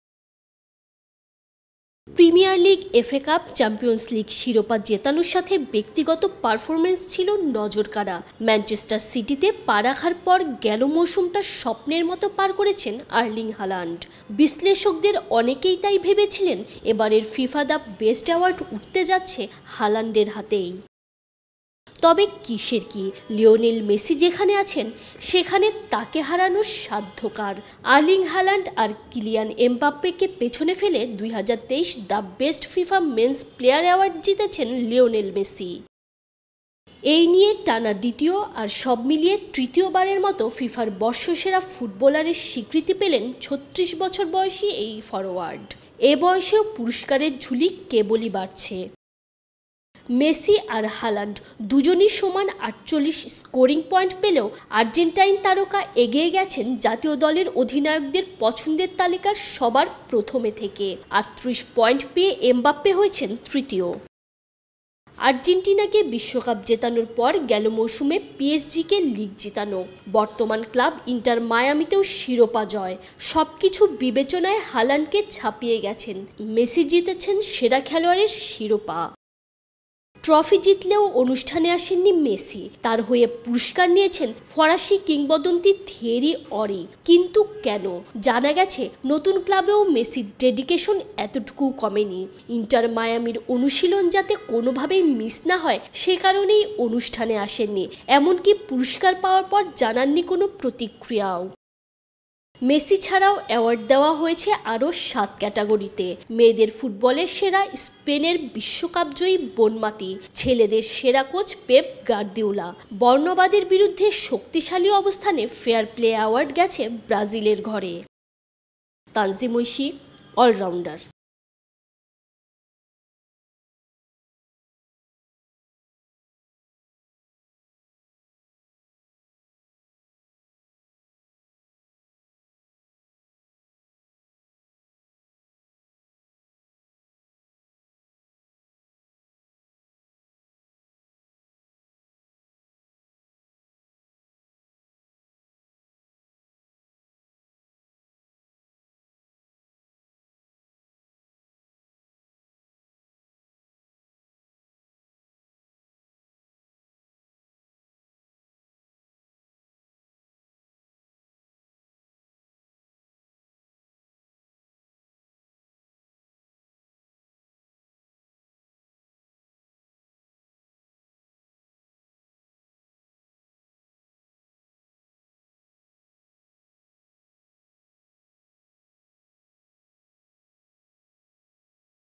অনুশীলন মিসের ভয়ে ফিফার অনুষ্ঠানেই এলেন না, জাতীয় দলের অধিনায়করাই জিতিয়ে দিলেন মেসিকে!
অনুশীলন মিসের ভয়ে ফিফার অনুষ্ঠানেই এলেন না মেসি, জাতীয় দলের অধিনায়করাই জিতিয়ে দিলেন আর্জেন্টাইন তারকাকে! তিন পুরস্কার গেল ব্রাজিলের ঘরে...